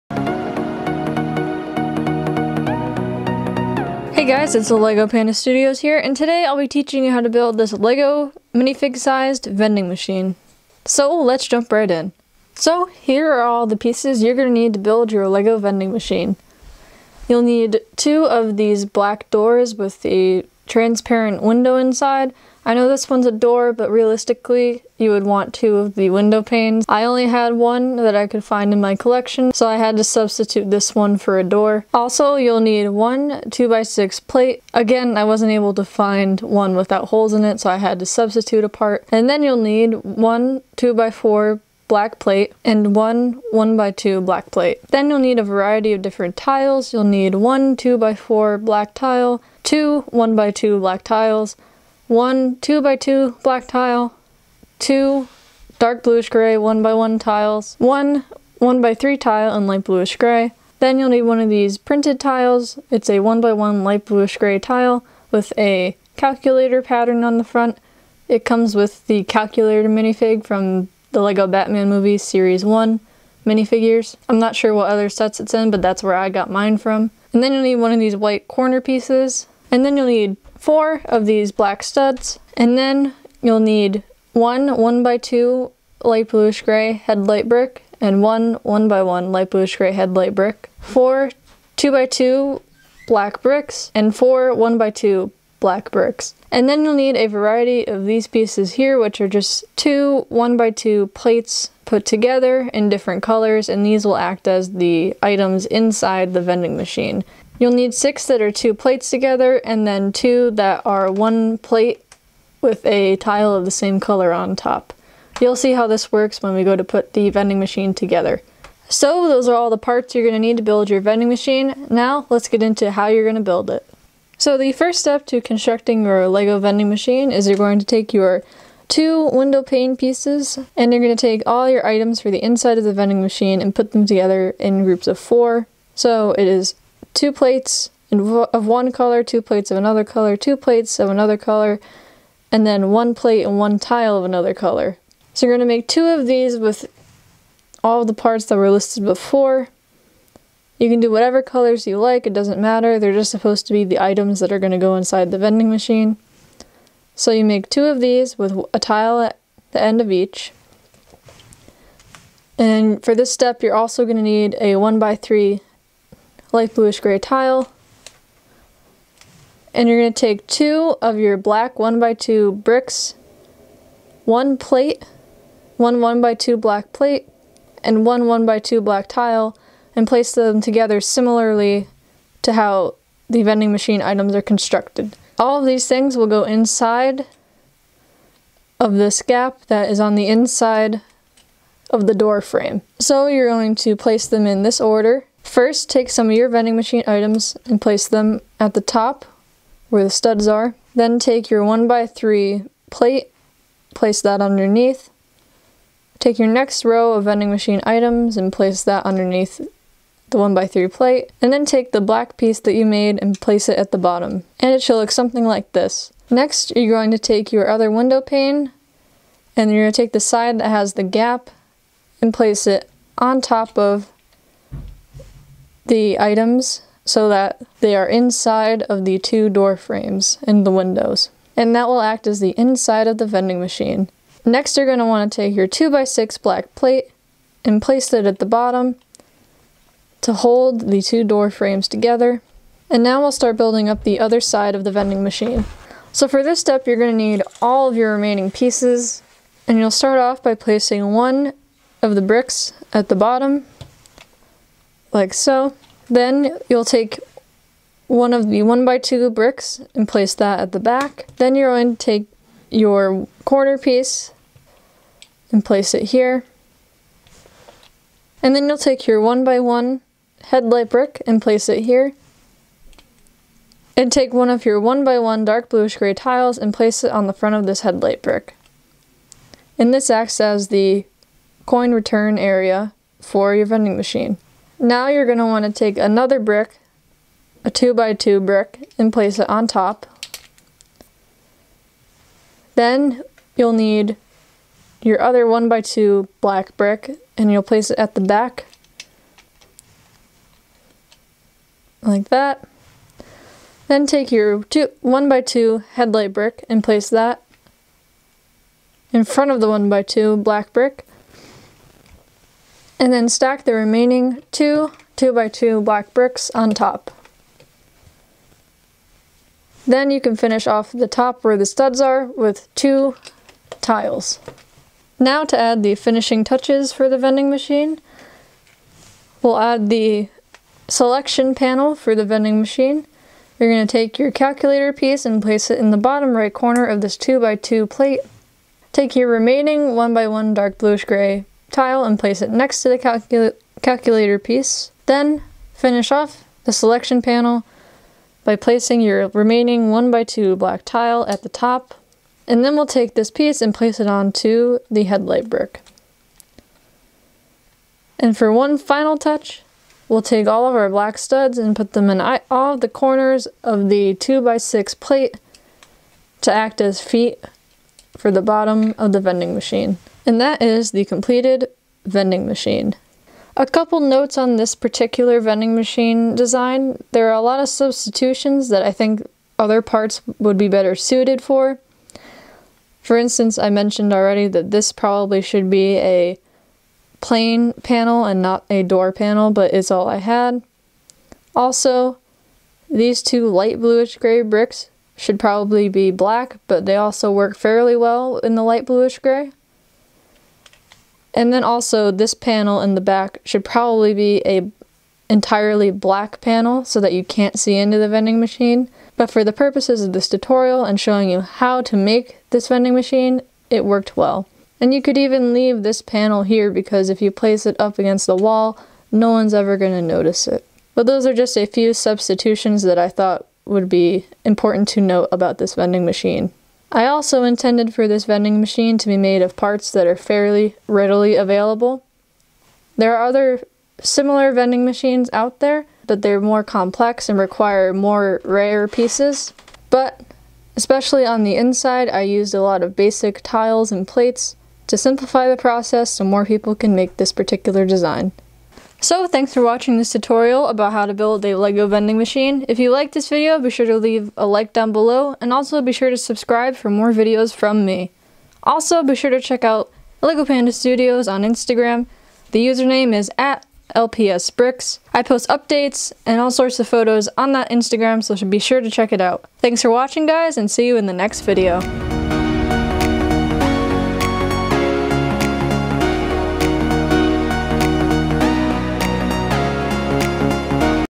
How to Build a LEGO Vending Machine | LEGO Tutorial
I show you how to build a simple LEGO vending machine, perfect for any LEGO MOC!